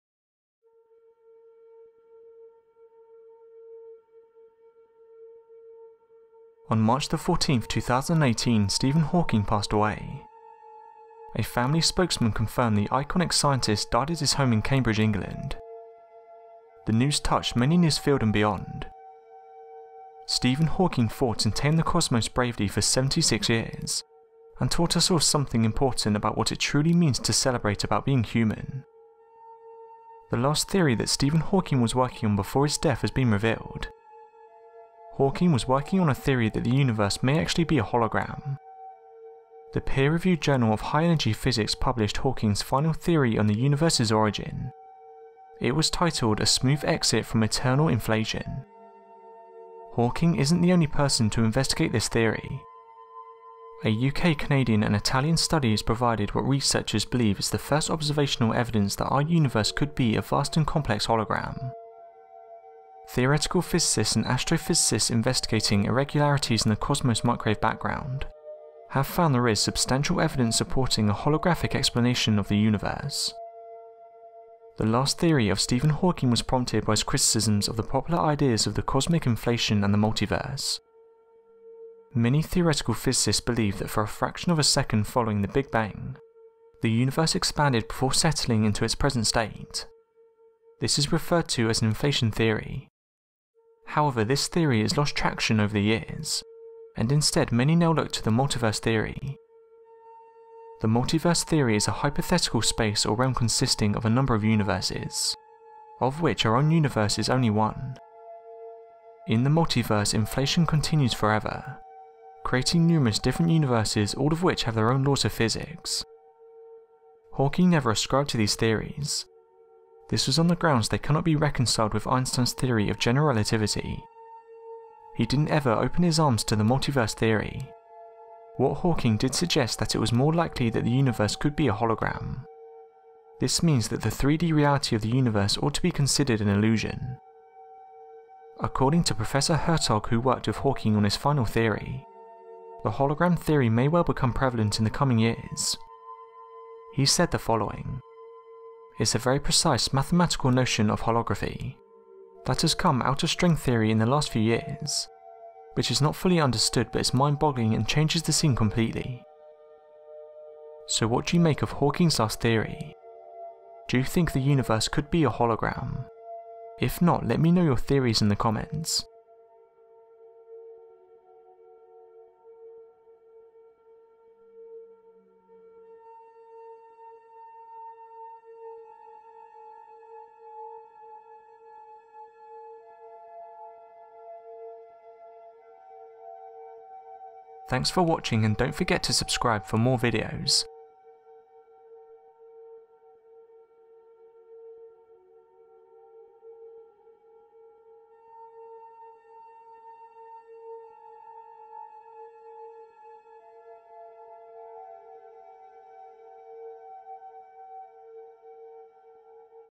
Before Stephen Hawking Passed Away He Made A Prediction
Before Stephen Hawking passed away he made a prediction. In this video we take a look at the last prediction Stephen Hawking made before he passed away.

The scientific world was left stunned when Stephen Hawking passed away. It's been revealed that the last theory Stephen Hawking was working on was a theory that the universe may actually be a hologram.